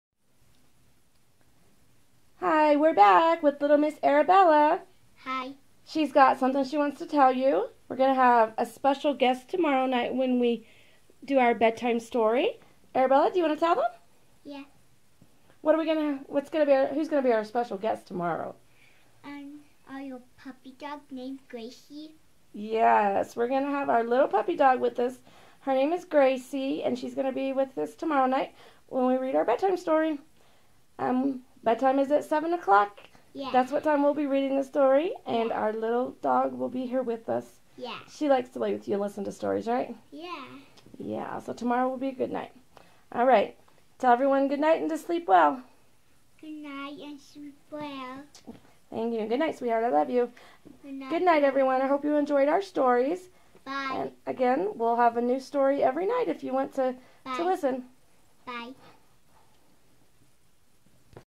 BEDTIME STORIES WITH ARABELLA!. Arabella announcing our special guest for tomorrow night!!!❤💤🐶
This is bedtime stories with Arabella! ❤❤💤 Every night we read a different bedtime story together. She wanted to do this together so other little boy's and girl's can hear the bedtime stories too! She is 5 years old and loves books. We hope you enjoy the videos. Goodnight from Arabella❤❤💤💤💤😴!